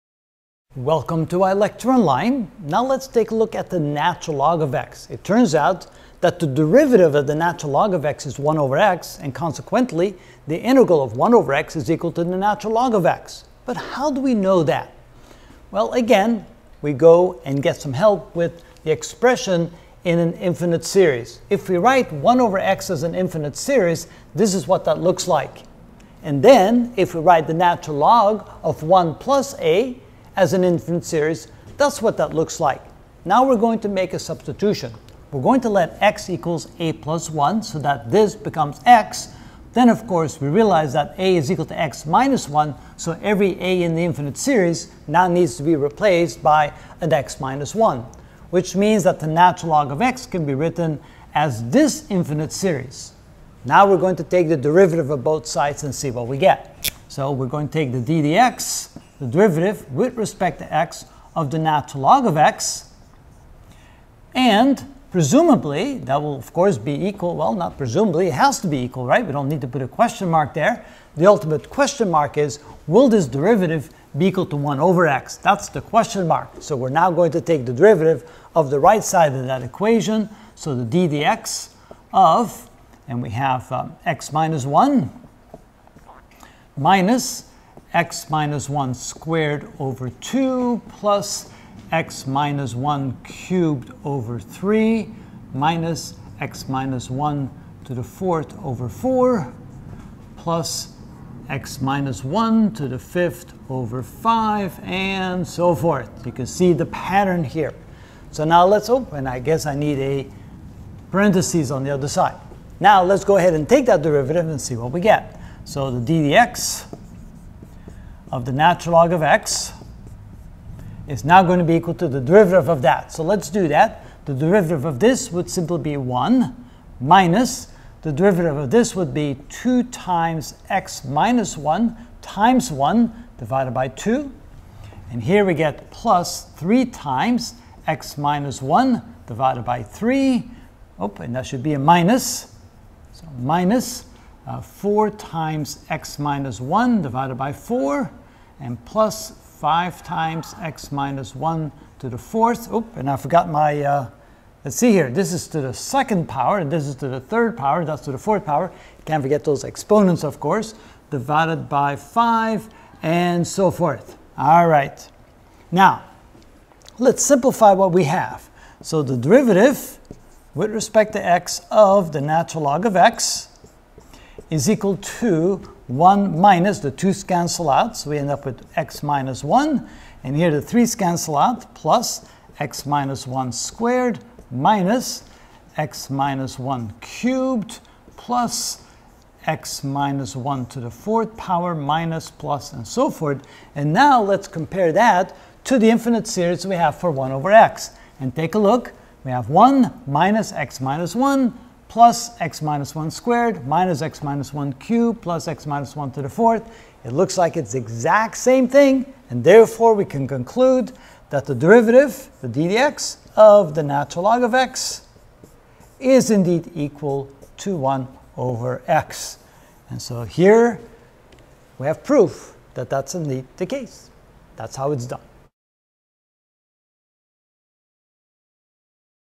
Calculus 1: Ch 5.1 Derivative of e^x and lnx (8 of 24) Why is the Derivative of ln(x)=1/x?
In this video I will use infinite series to show why is the derivative d/dx(ln(x))=1/x.